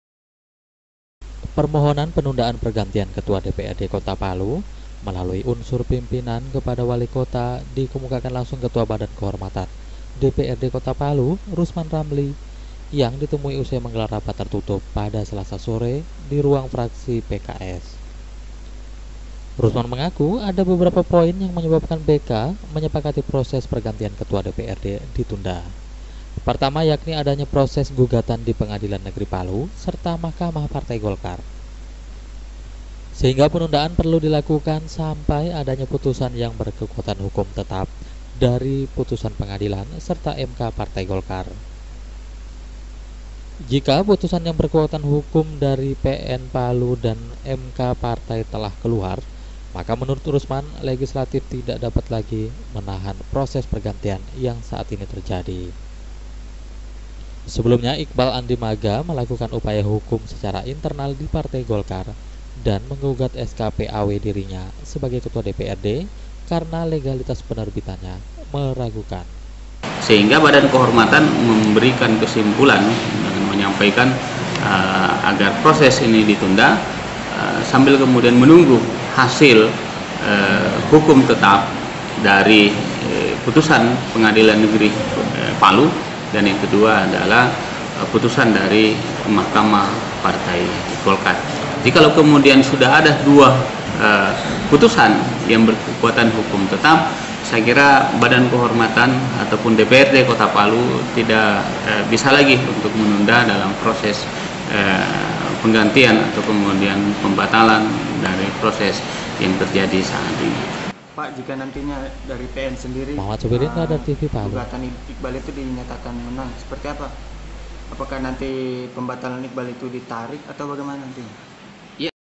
BK DPRD Kota Palu akan Ajukan Penundaan Pergantian Ketua
Dewan Perwakilan Rakyat Daerah Kota Palu ( DPRD Kota Palu ), dalam berencana mengajukan permohonan penundaan penerbitan SK pergantian Ketua DPRD Kota Palu kepada Wali Kota. Hal ini berdasarkan hasil rapat Badan Kehormatan atas surat masuk dari kuasa hukum Iqbal Andi Magga.